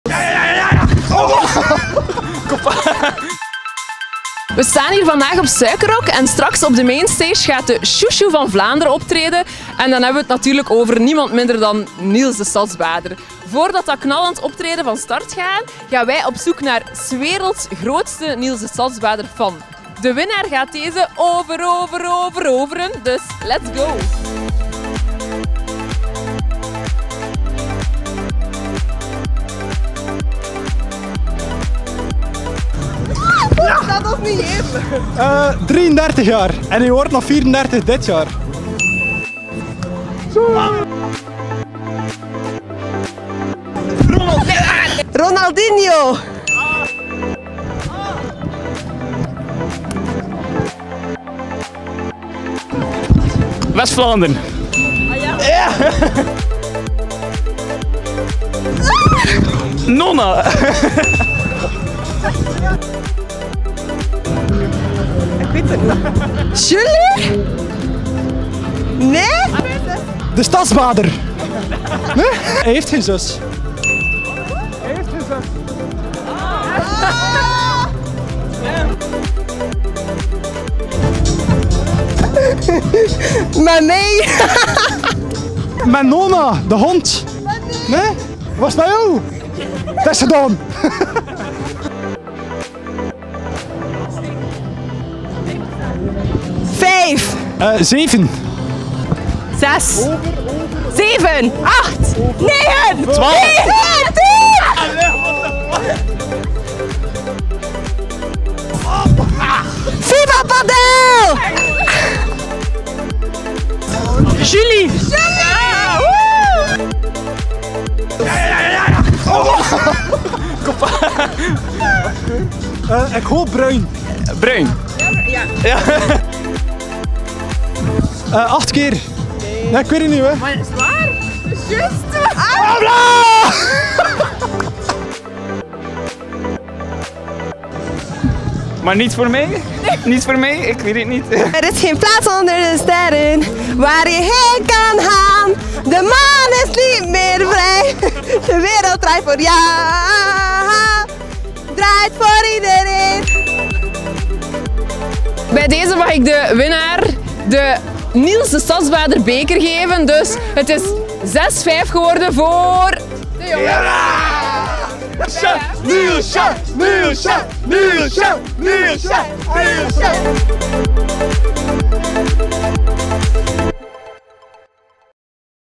ZIEN: Flair zocht ’s werelds grootste Niels Destadsbader-fan op Suikerrock
Het ging er razend spannend aan toe in een toffe quiz op het festivalterrein.

Voor meer info, ga naar Flair.be